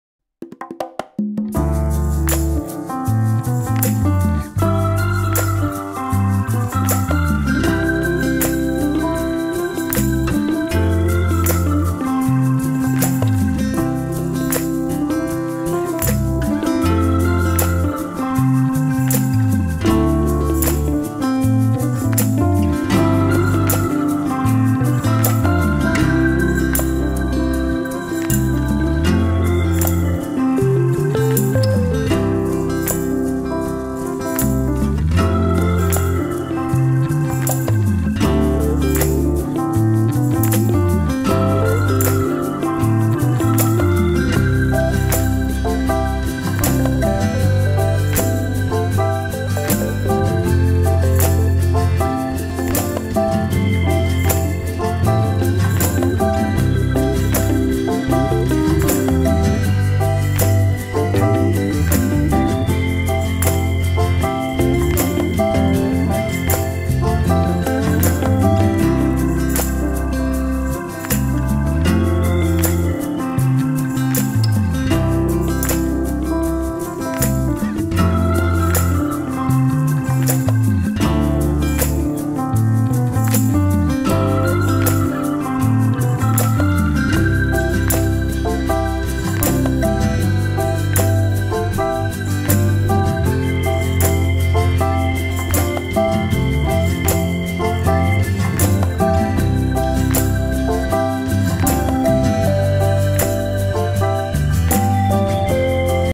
Quotes Poster Design In Just 2 Minutes
How to make Quotes poster in just 2 minutes using Photoshop CS6